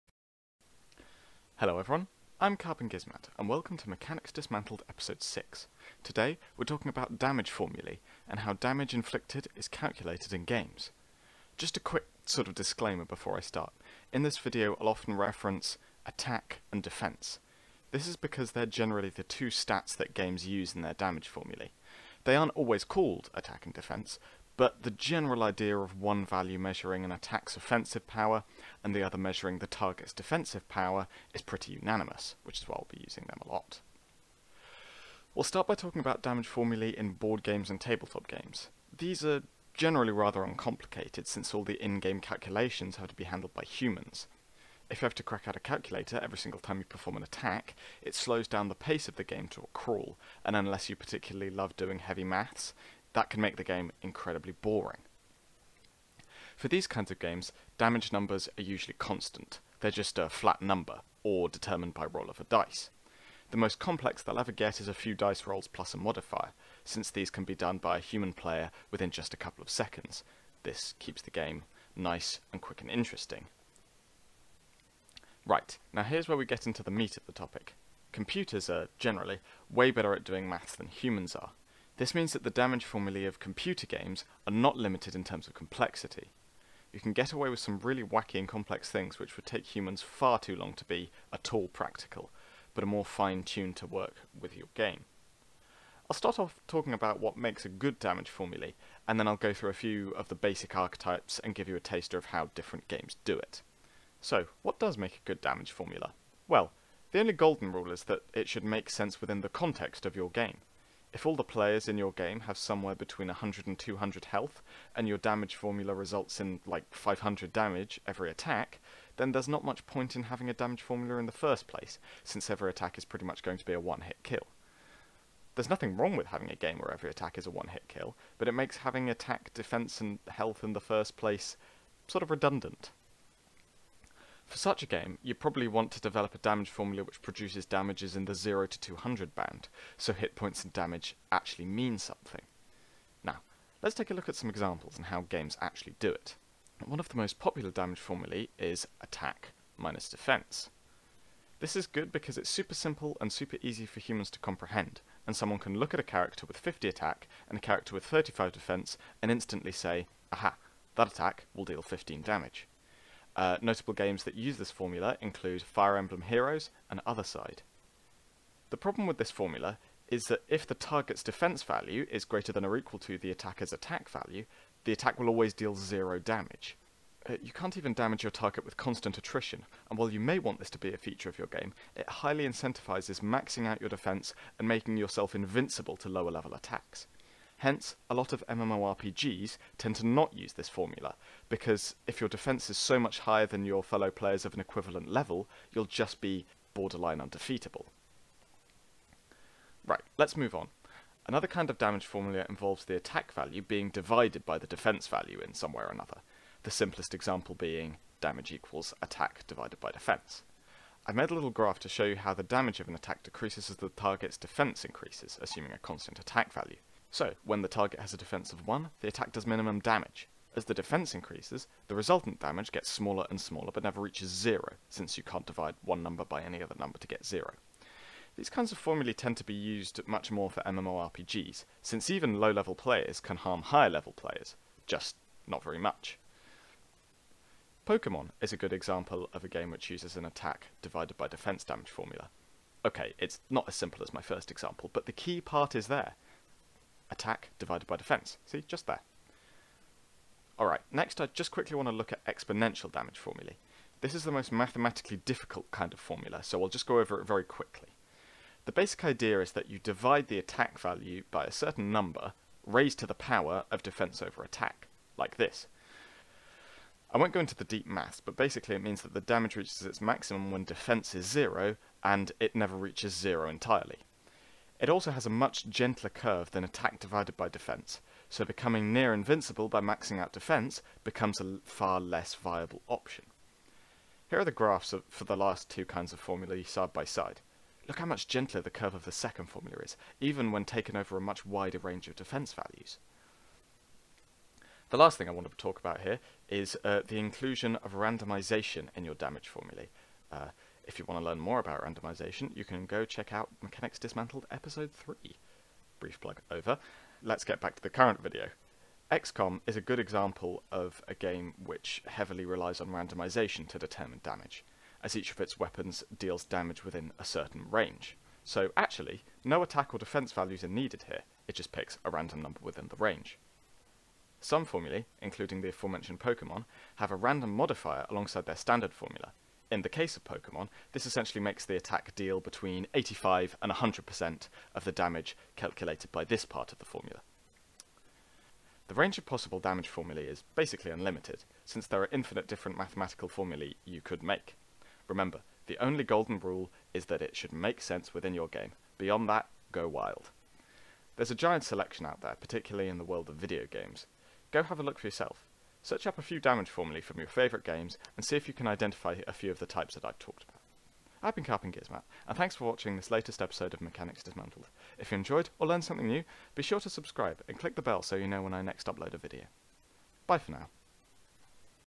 How do video games calculate damage? || Mechanics Dismantled 6
Breaking down the different ways that damage is calculated in games, and why those formulae are used the way they are.